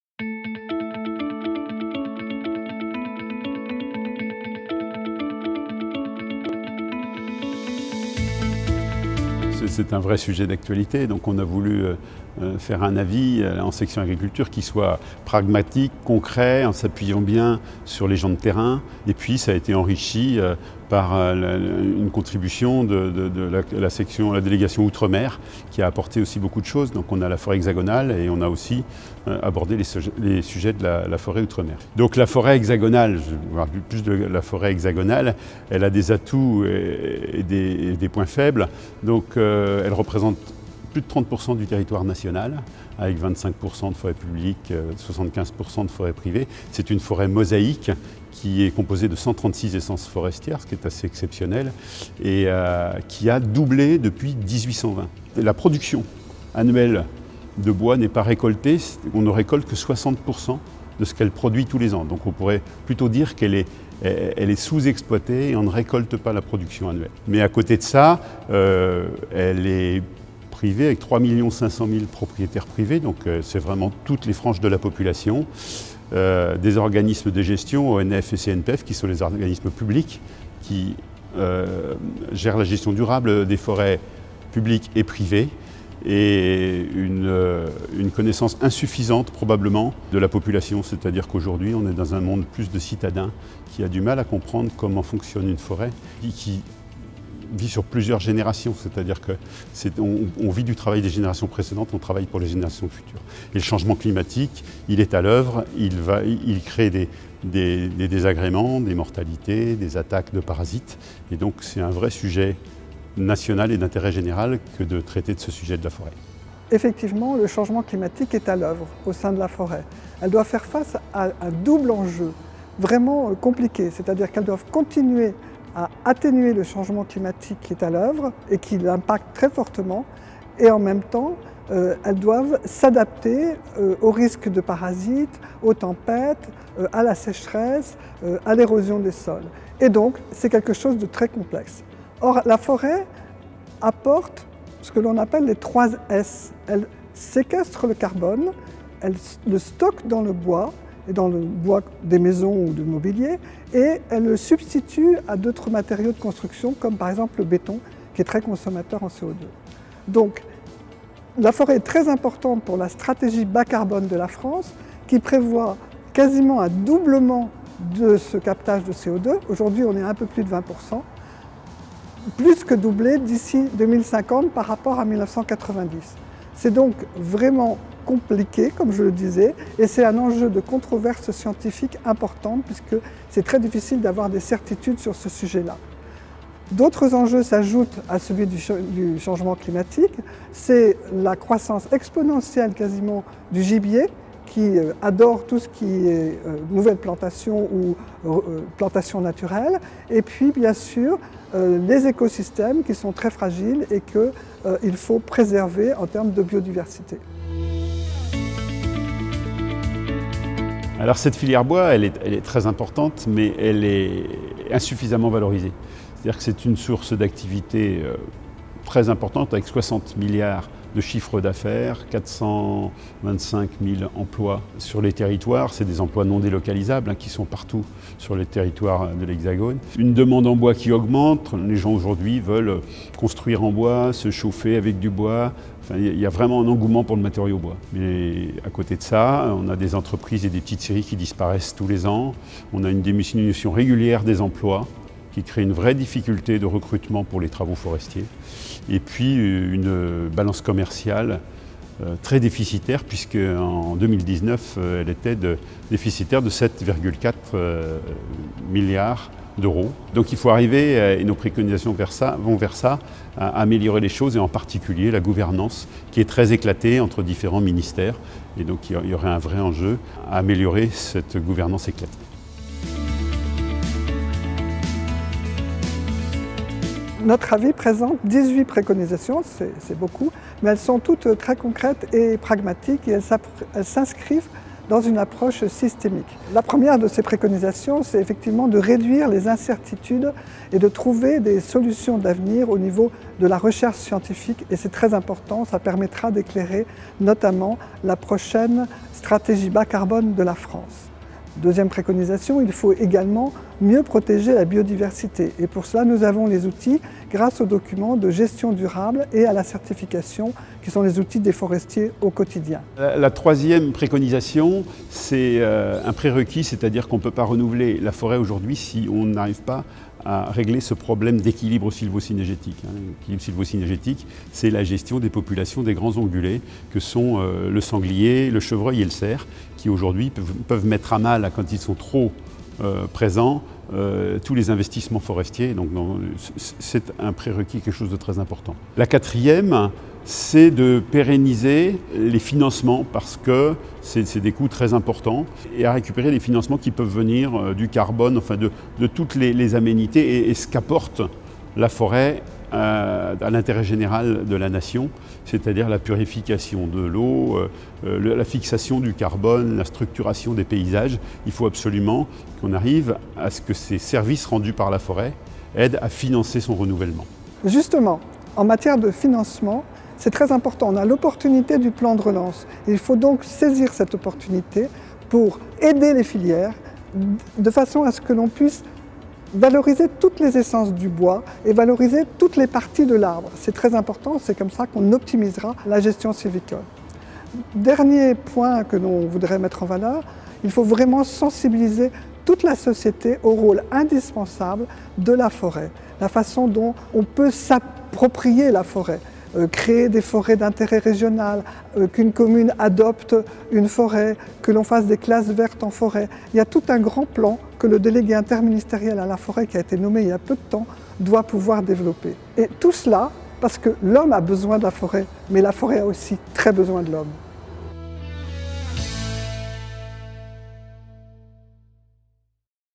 Face au changement climatique, quelle sylviculture durable ? - cese
Avis présenté par Marie-Hélène Boidin Dubrule (groupe des entreprises) et Antoine d'Amécourt (personnalité associée), au nom de la section de l'agriculture, de la pêche et de l'alimentation, présidée par Etienne Gangneron.

Les forêts françaises hexagonales et ultramarines, vastes et diversifiées constituent des atouts indéniables pour notre pays à de nombreux titres : vie économique, emplois, richesse de biodiversité, captation et stockage de carbone, paysages, loisirs…
Or, le changement climatique a déjà commencé, dans certaines régions, à porter atteinte à ce patrimoine indispensable à l’avenir de la société.
Il s’agit par conséquent d’intensifier la contribution des forêts à la lutte contre le changement climatique tout en les adaptant dans l’objectif de leur plus grande résilience.

Compte tenu de l’urgence de réagir face à ces défis, le CESE propose des actions concrètes qui s’appuient sur l’expérience des acteurs du secteur forêt- bois, pour proposer une sylviculture durable. Leur mise en œuvre doit mobiliser propriétaires et gestionnaires forestiers, pouvoirs publics, organismes de recherche, associations mais aussi les citoyens eux-mêmes. En effet, au regard de l’importance et de la diversité des services qu’elles rendent, l’avenir des forêts est l’affaire, l’Homme et la forêt ayant besoin l’un de l’autre !